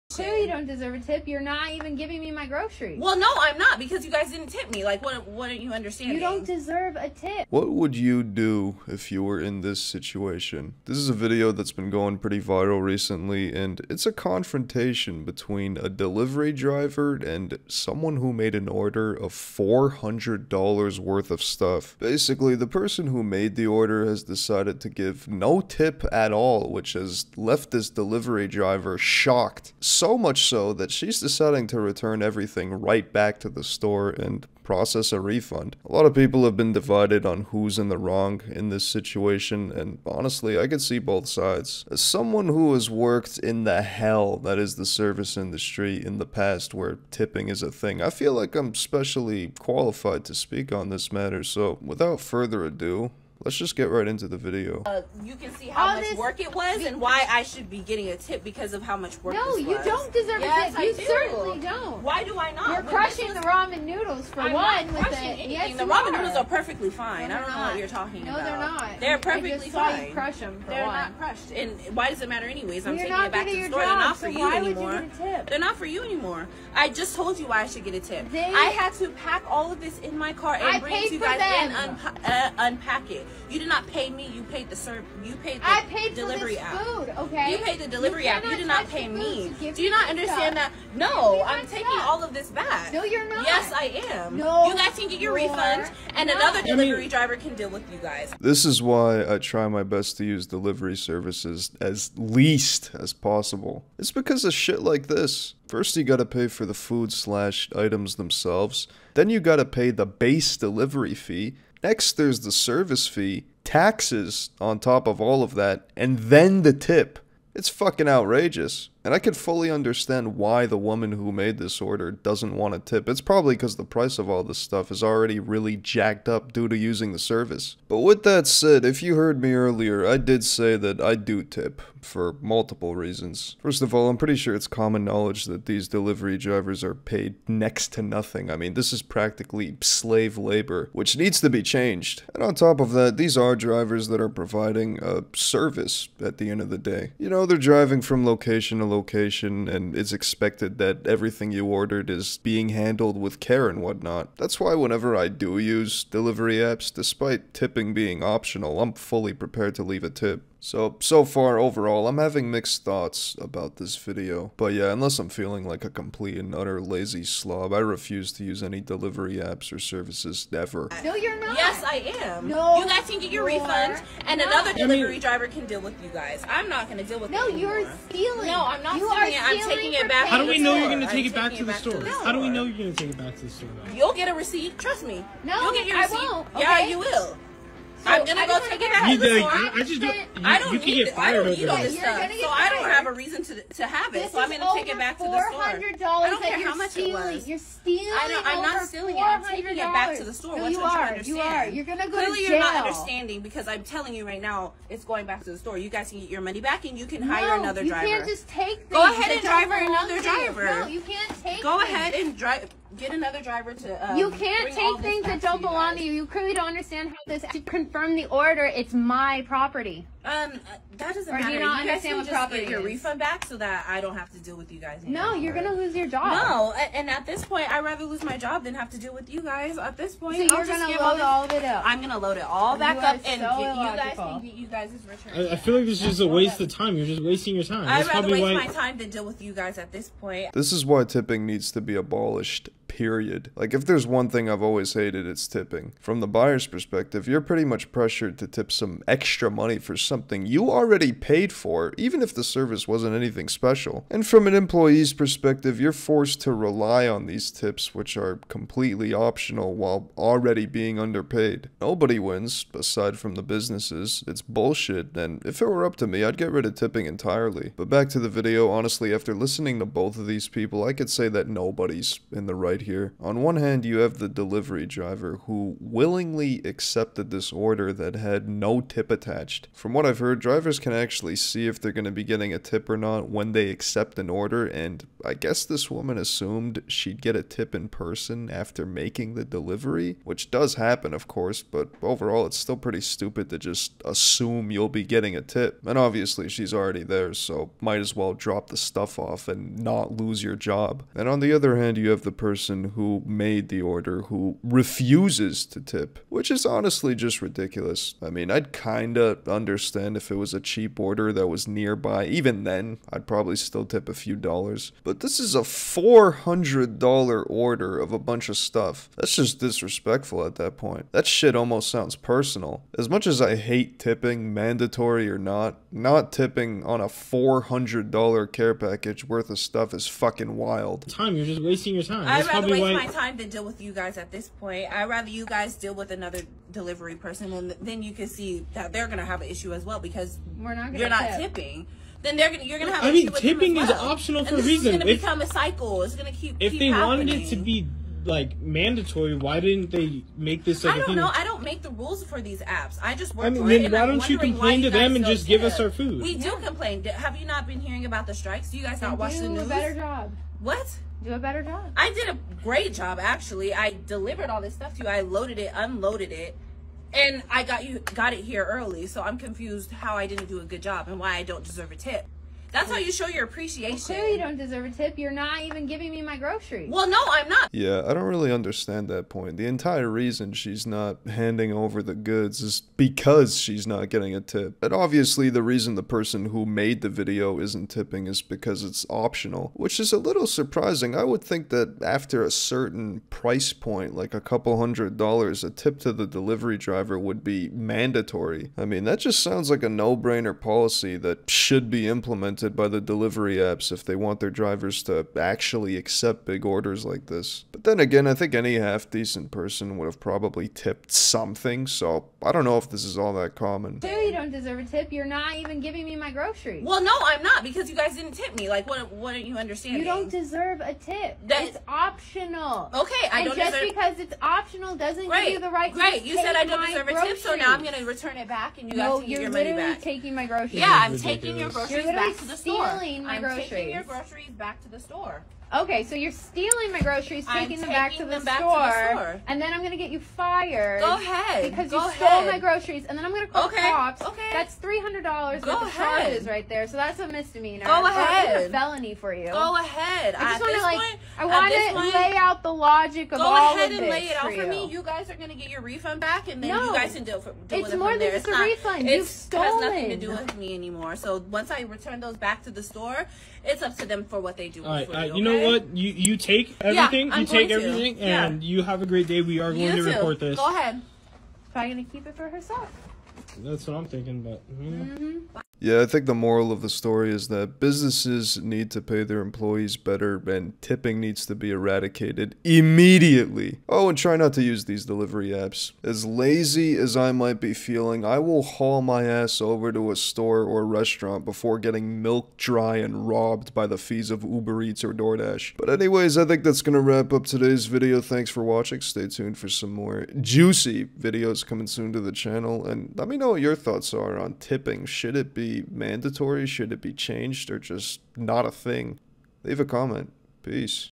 Delivery Driver Gets No Tip, REFUSES To Give $400 Order
Today we're gonna be taking a look at recent situation that went viral where a delivery driver decided to bring everything back after no tip on a $400 order